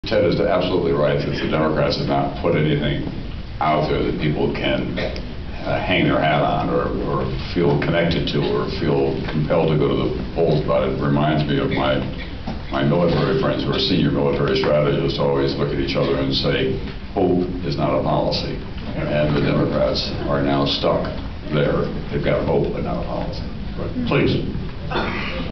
Tom Brokaw on the Democrats' Chances in 2006
Tom Brokaw at a forum in Dallas, Texas, September 26, 2006.